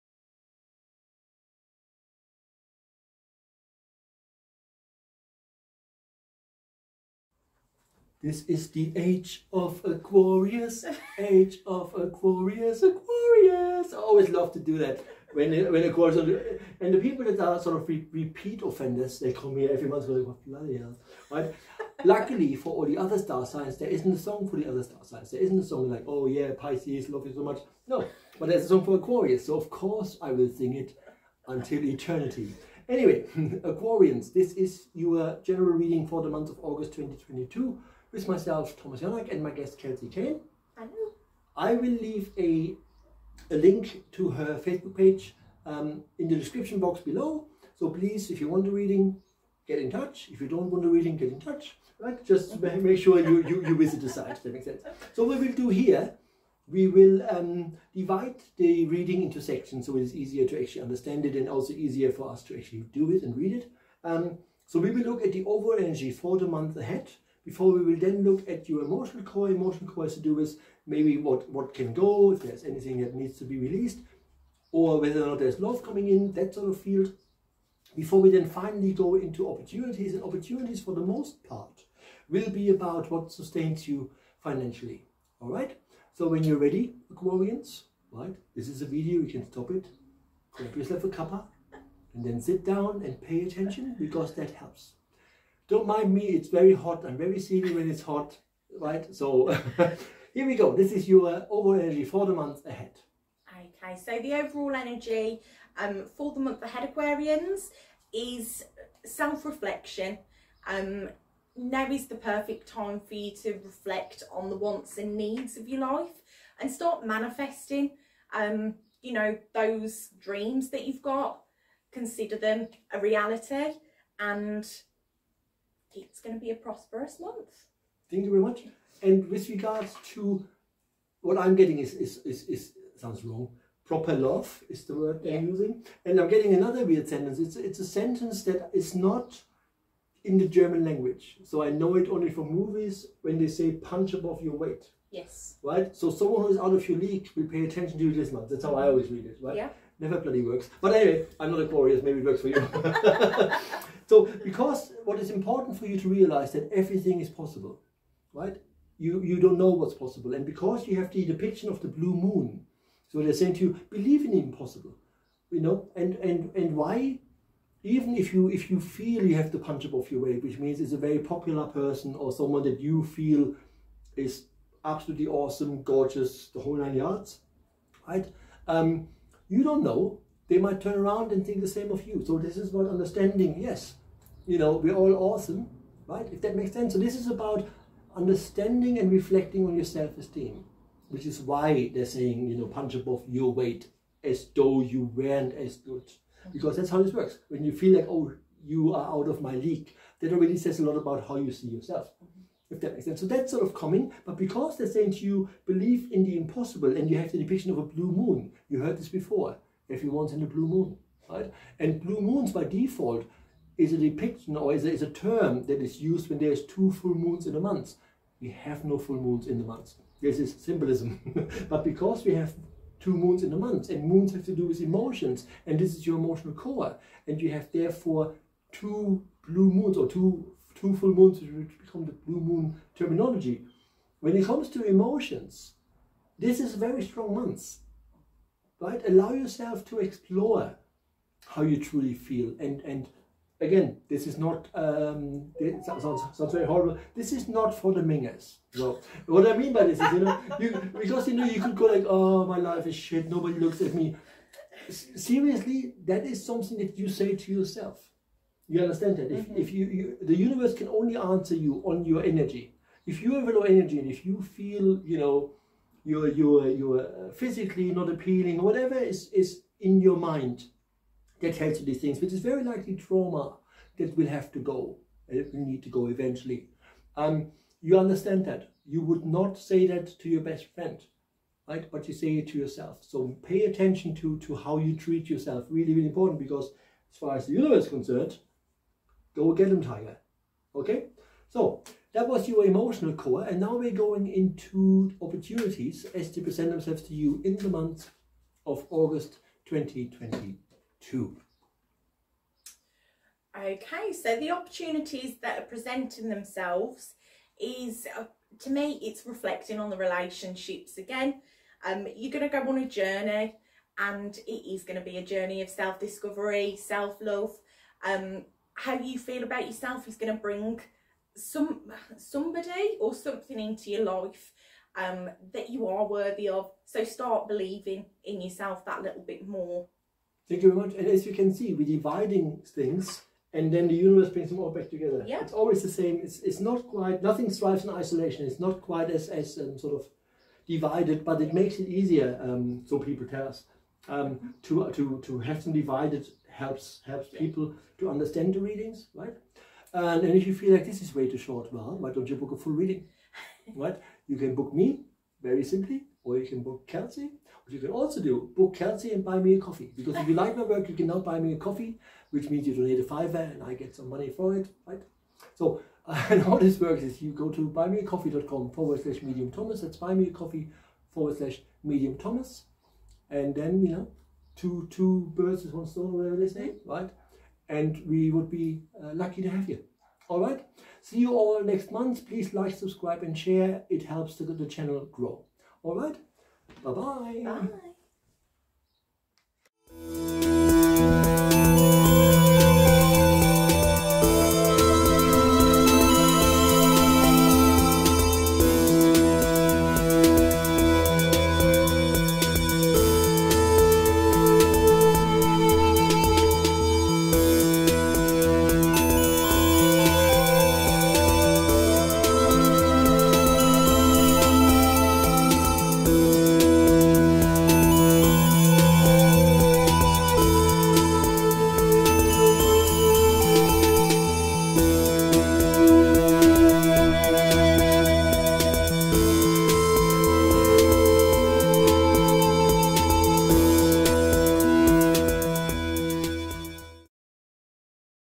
AQUARIUS AUGUST 2022 GENERAL READING WITH GUEST READER KELSEY CANE
Self reflection is your task this month - and there is so much more.